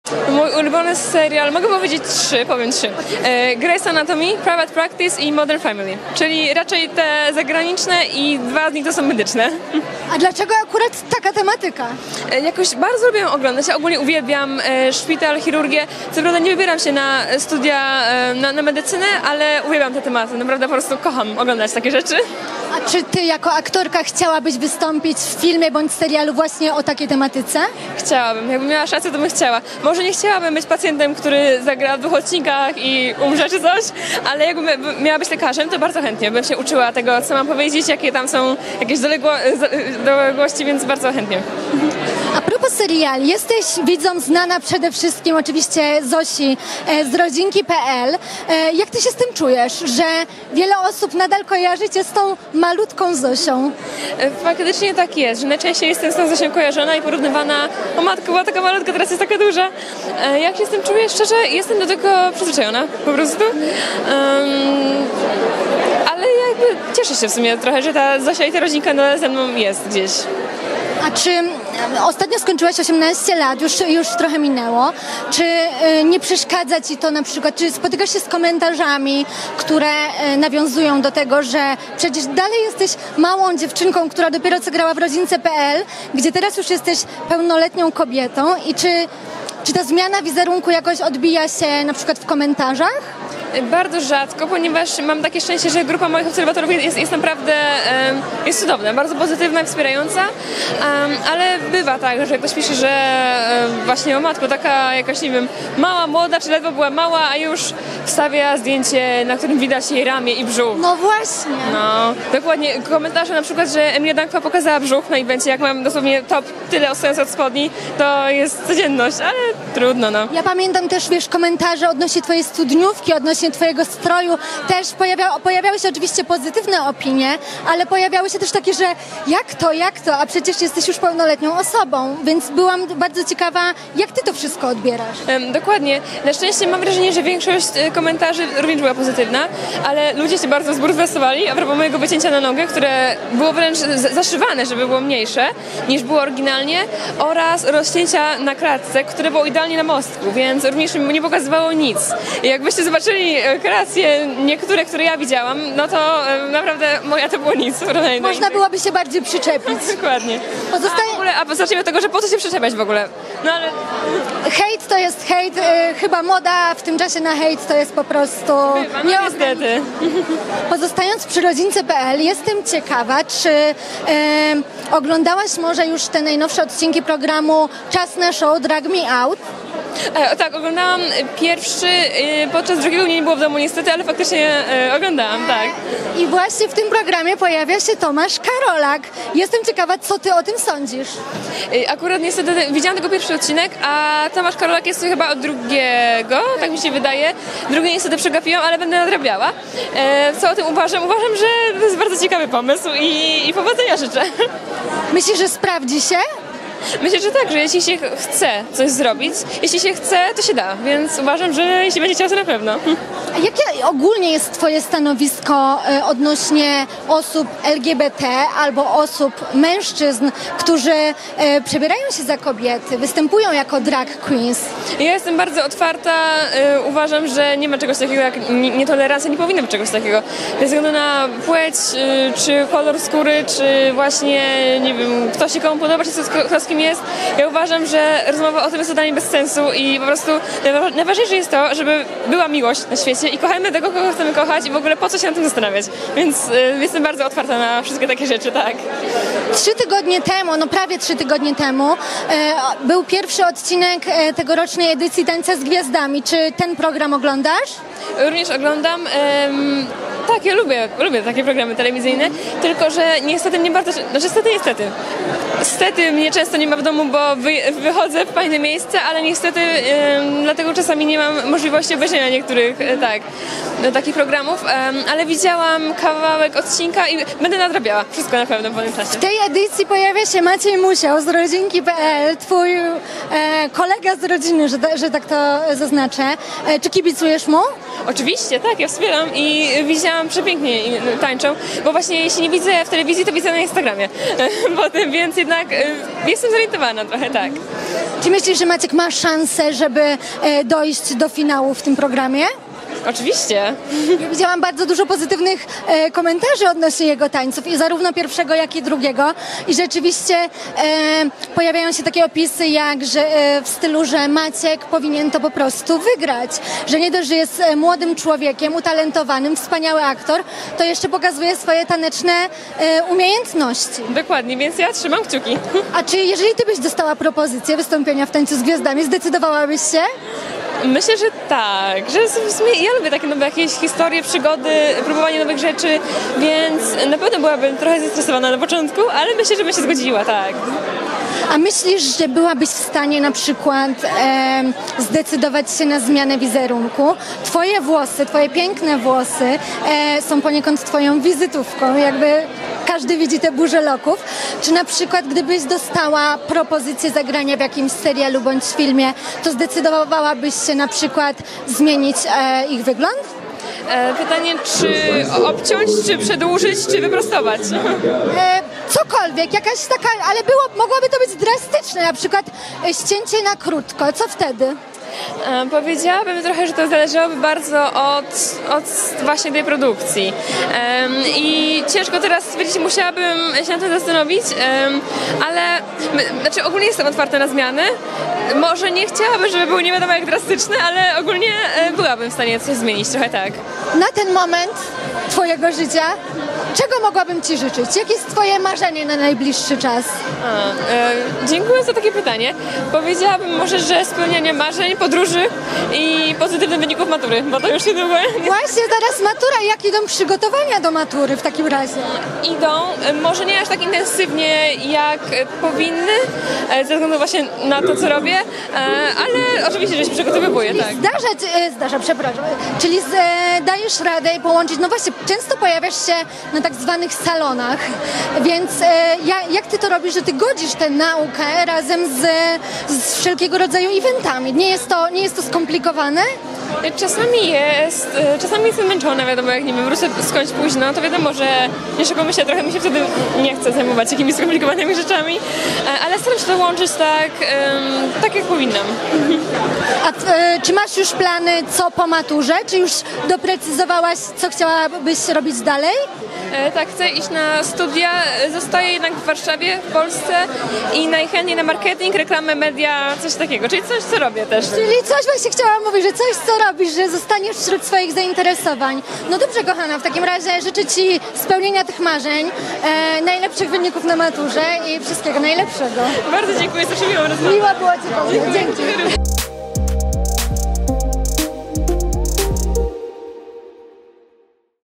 WOW! Emilia Dankwa o Rodzinka.pl, LGBT, Tomku Karolaku w "Czas na Show Drag Me Out" i HEJCIE
Wywiad z Emilia Dankwa, Top Seriale 2024,, Warszawa -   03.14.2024 r.